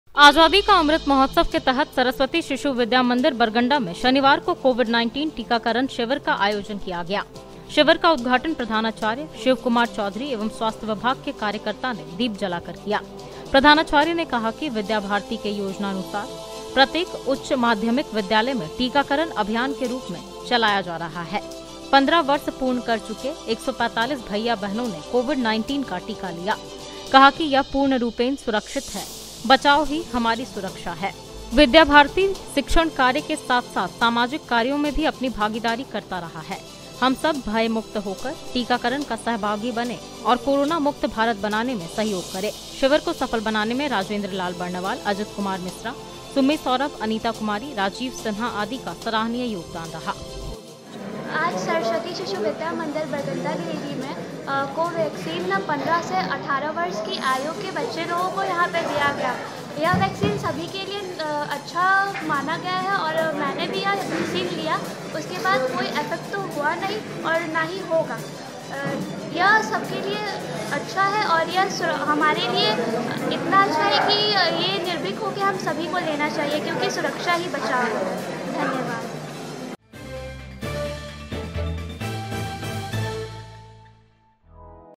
Giridih आजादी का अमृत महोत्सव के तहत SSVM में कोविड टीकाकरण शिविर का आयोजन
आजादी का अमृत महोत्सव के तहत सरस्वती शिशु विद्या मंदिर में कोविड टीकाकरण शिविर का आयोजन किया गया जिसमे 15 वर्ष से अधिक के छात्रों ने टिका लिया।मौके पर प्रधानाचार्य ने सभी को कोविड वैक्सीन लेने के लिए जागरूक भी किया।